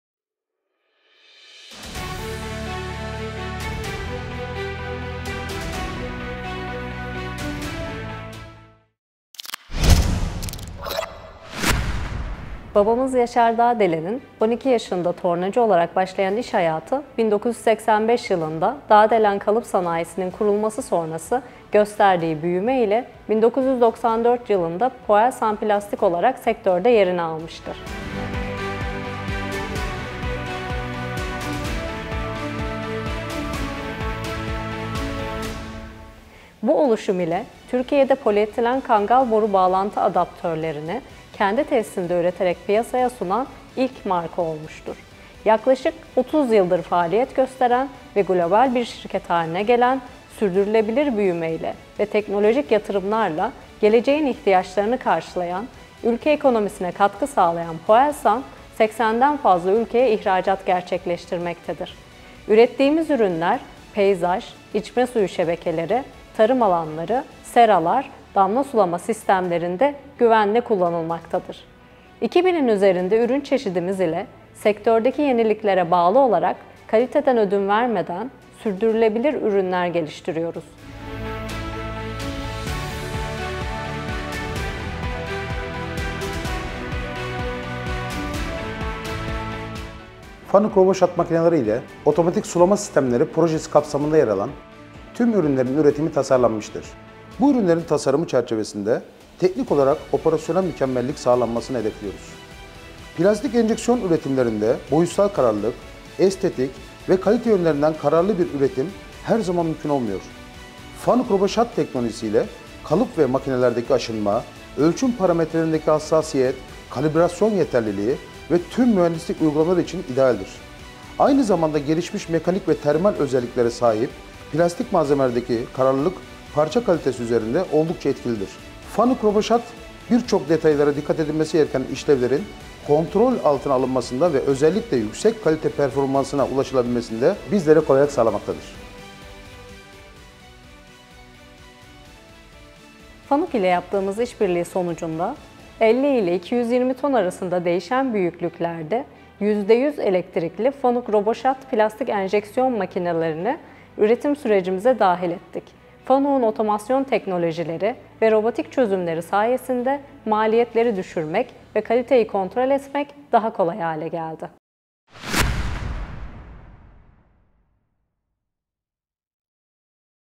Cut costs and boosts quality with ROBOSHOT machines | Poelsan Case Study
Poelsan faced the challenge of meeting various demands, including dimensional stability, high aesthetics, productivity, low energy consumption, and maintenance. In search of a suitable injection molding machine partner, they found FANUC's ROBOSHOT α-SiB machines.

Now, with several in operation at their advanced factory, Poelsan processes 10,500 tonnes of plastic annually, achieving operational excellence in cost and quality. The all-electric operation, low total cost of ownership, and high precision of the ROBOSHOT machines contribute to the company's success, providing benefits like energy savings and minimal maintenance.